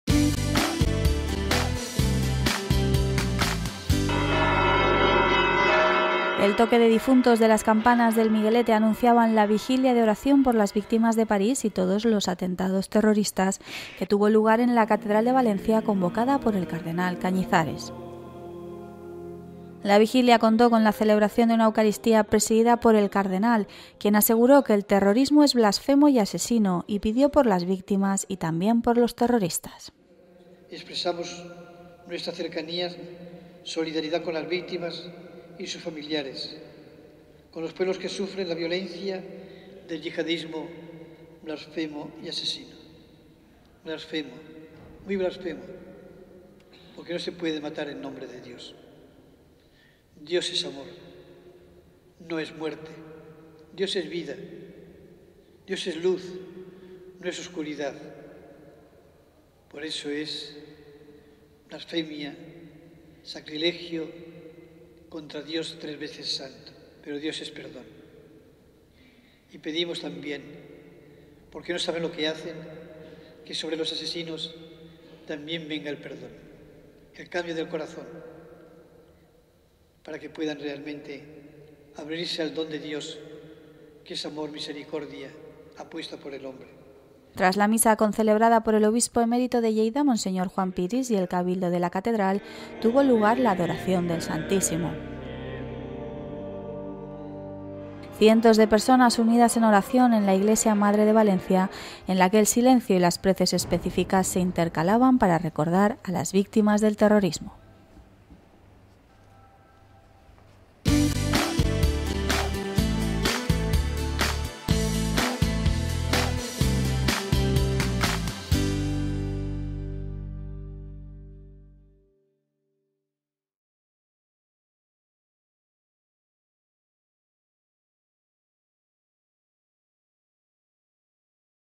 Fieles valencianos se unen en oración por las víctimas del terrorismo yihadista
En la misa que presidió el cardenal Cañizares en la Catedral de Valencia, dentro de la vigilia de oración convocada por las víctimas de los atentados terroristas, aseguró que "Jesucristo no es espectador de este escarnio, sino que lo sufre en su propia carne".